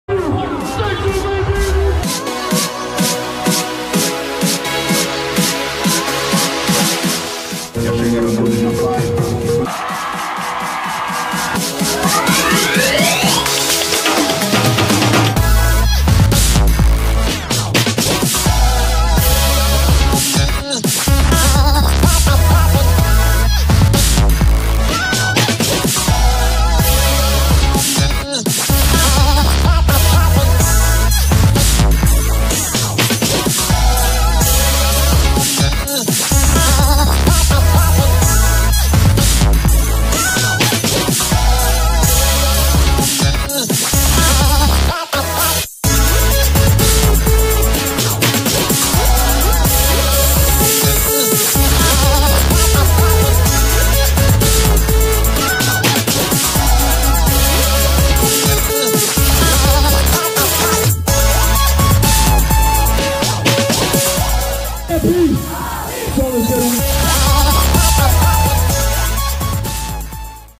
Happy Holi 2019
A música eletrónica pintou-se de cor, este domingo, em Lisboa, no festival Happy Holi.